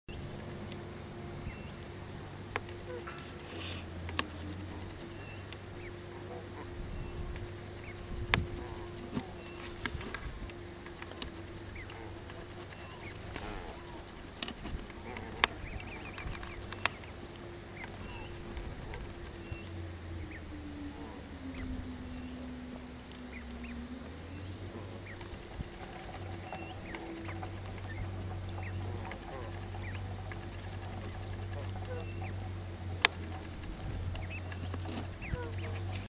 Happy hopping Barnacle goose juvenile
Unable to fly, the three day old goslings jump off the cliff and fall; their small size, feathery down, and very light weight helps to protect some of them from serious injury when they hit the rocks below, but many die from the impact. Arctic foxes are attracted by the noise made by the parent geese during this time and capture many dead or injured goslings. The foxes also stalk the young as they are led by the parents to wetland feeding areas. - Wikipedia.
The  Barnacle geese feeding on sprouts of grass and plants.